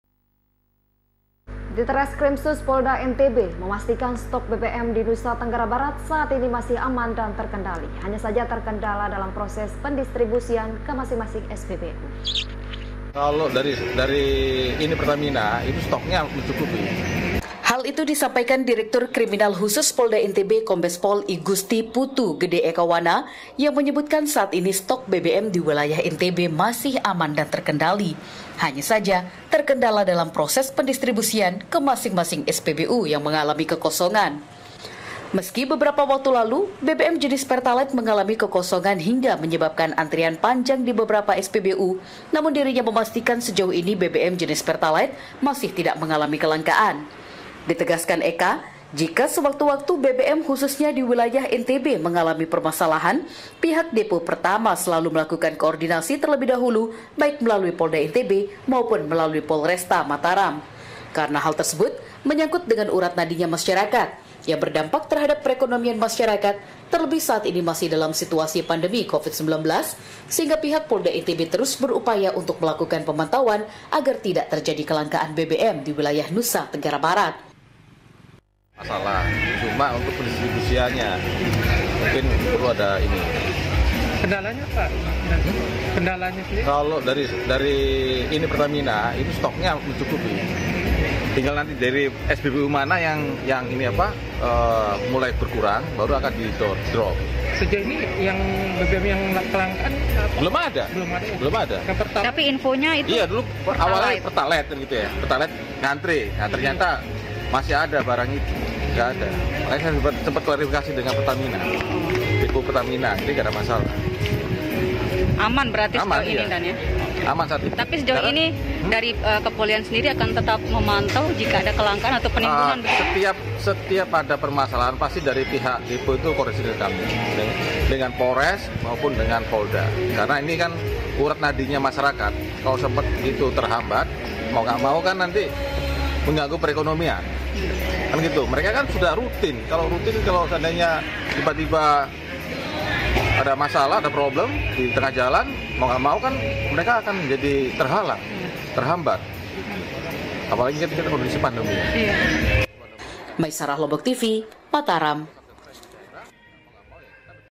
DITRESKRIMSUS POLDA NTB PASTIKAN STOK BBM AMAN
DITRESKRIMSUS POLDA NTB MEMASTIKAN STOK BBM DI NUSA TENGGARA BARAT SAAT INI MASIH AMAN DAN TERKENDALI HANYA SAJA TERKENDALA DALAM PROSES PENDISTRIBUSIAN KE MASING MASING SPBU//